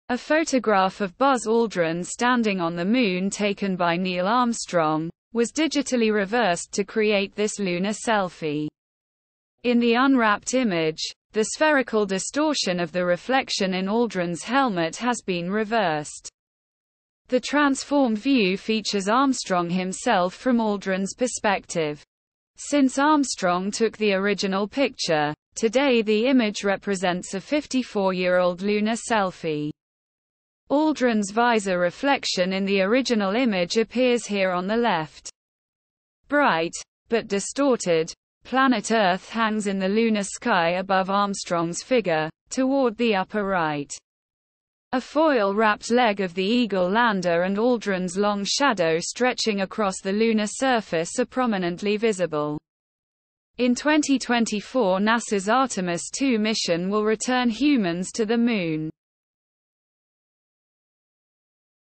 Apollo 11: Armstrong's Lunar Selfie
A photograph of Buzz Aldrin standing on the Moon taken by Neil Armstrong, was digitally reversed to create this lunar selfie. Captured in July 1969 following the Apollo 11 moon landing, Armstrong's original photograph recorded not only the magnificent desolation of an unfamiliar world, but Armstrong himself reflected in Aldrin's curved visor.  In the unwrapped image, the spherical distortion of the reflection in Aldrin's helmet has been reversed.  The transformed view features Armstrong himself from Aldrin's perspective. Since Armstrong took the original picture, today the image represents a fifty-four year old lunar selfie.  Aldrin's visor reflection in the original image appears here on the left. Bright (but distorted) planet Earth hangs in the lunar sky above Armstrong's figure, toward the upper right. A foil-wrapped leg of the Eagle lander and Aldrin's long shadow stretching across the lunar surface are prominently visible.  In 2024 NASA's Artemis II mission will return humans to the Moon.